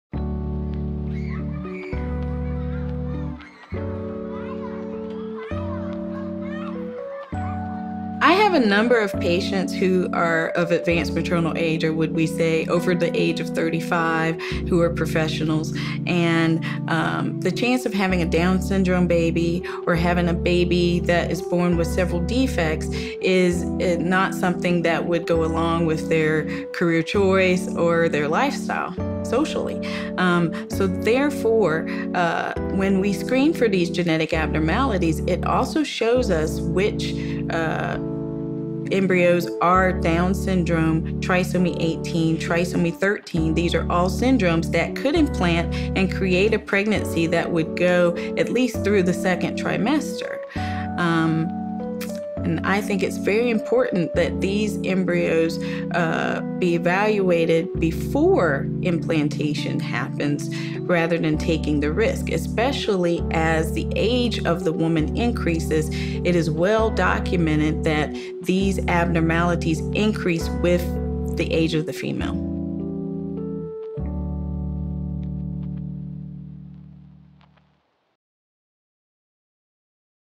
Risks of In Vitro Fertilization after 35
- Dr. Eva Littman talks about how women over 35 can still use In Vitro Fertilization, combined with genetic screening to successfully have a child.  Find out what risks and problems you may face by watching this video.